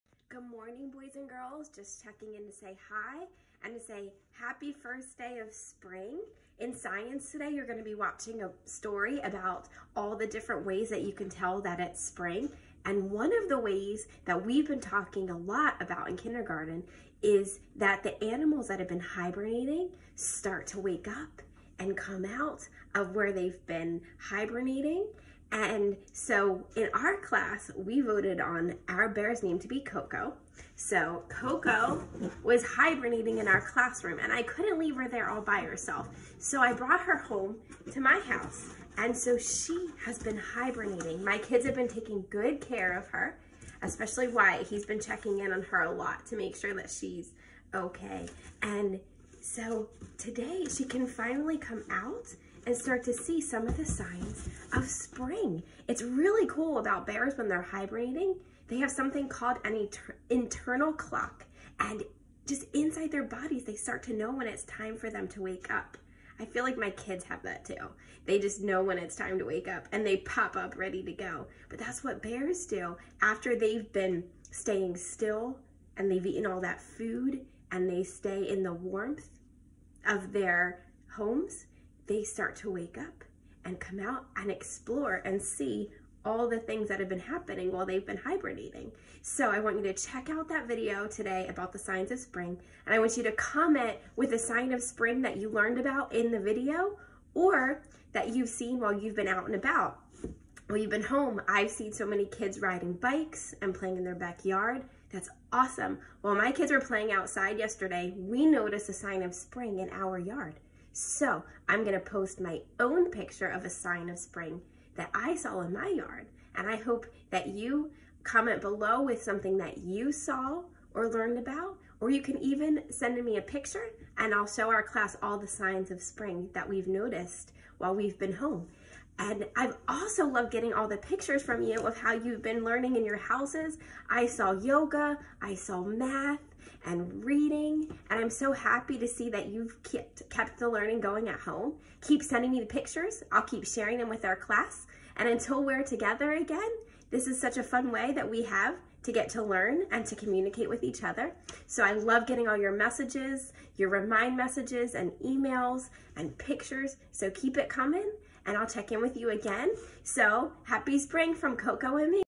Cocoa Comes Out of Hibernation
Happy first day of Spring! Our class bear can finally come out of hibernation!